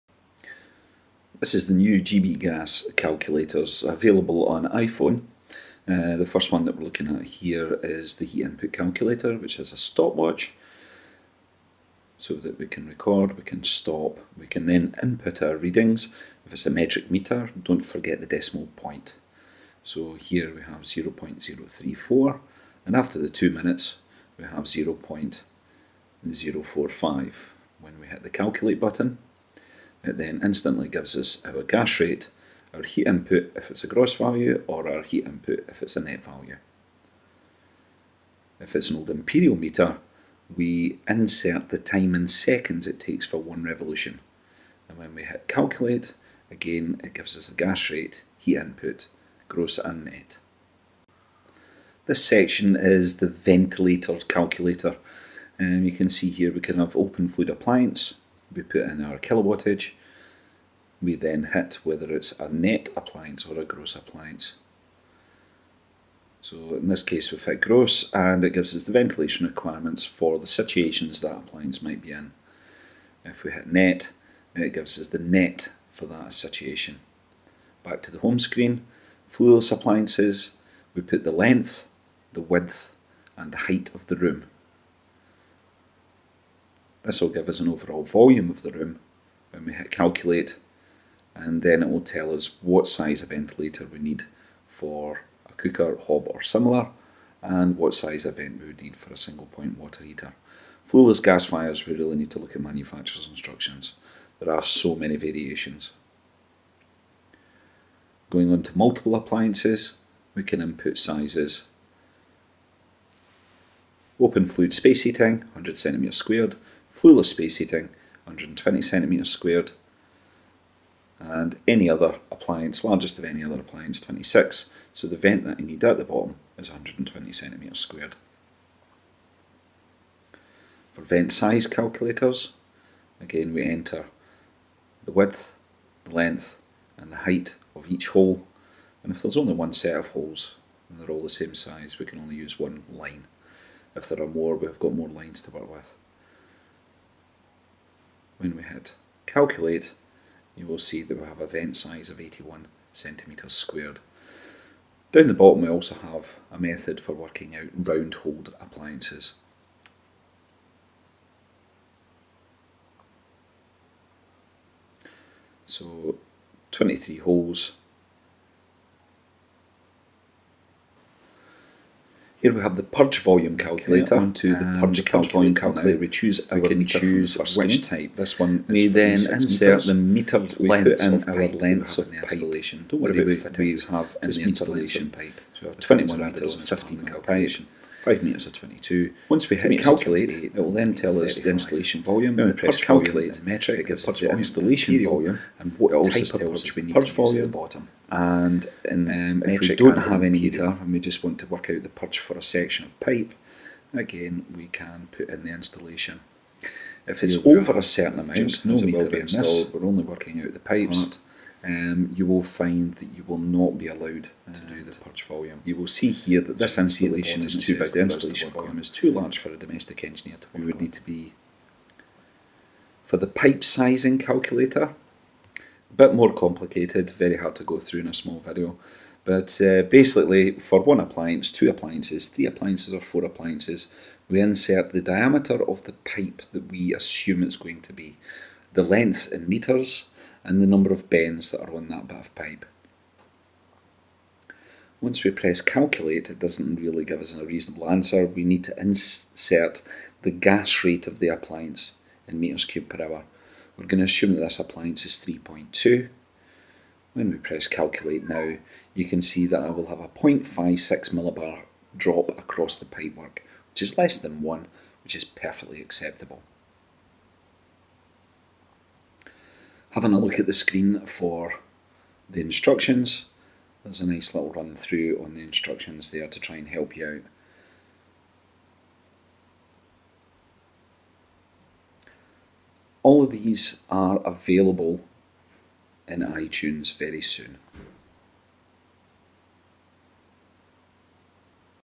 GB-Gas Iphone Calculator Apps
I have been developing gas calculator style apps for the Iphone. As of today (upload date) the apps are waiting to be verified by itunes.
There are four different apps at this stage:
Heat Input / Gas rate Calculator,
Purge rate Calculator,
Ventilation Calculator,
Pipe Size Calculator.

Check out the video for a bit of tuition and see how easy it is to complete the difficult equations and in a fraction of the time. These are a must for gas installers.

Yes... i am working on other phone versions.  follow gbgas001 on twitter